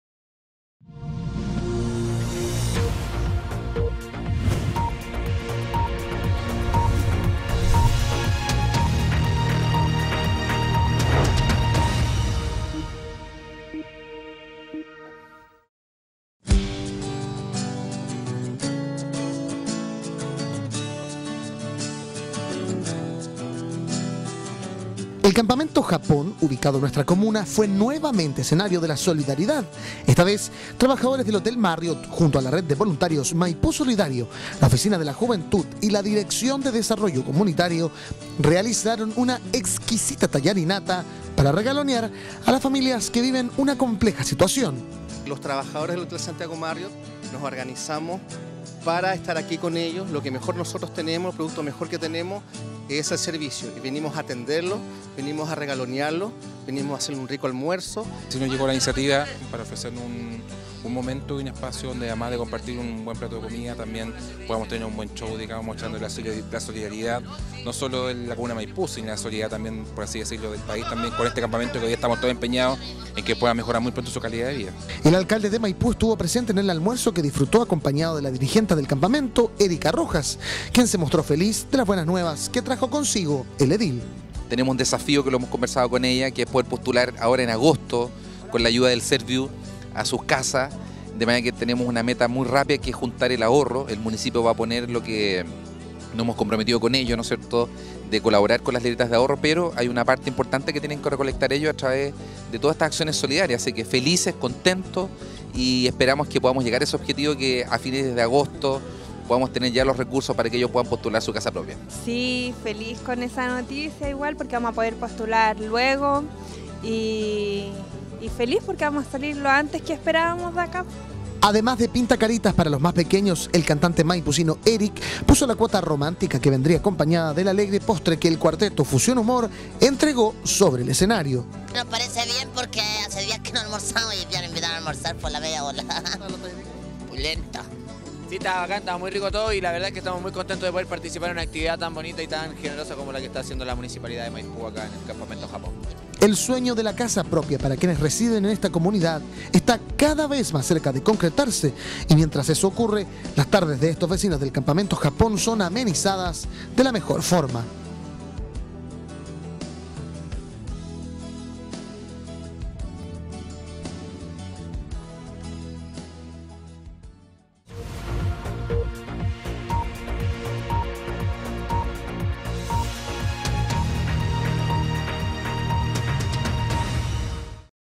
TALLARINATA ENSALZÓ ALEGRIA
Una exquisita tallarinata fue la que disfrutaron vecinos del campamento Japón quienes además gozaron de un alegre espectáculo.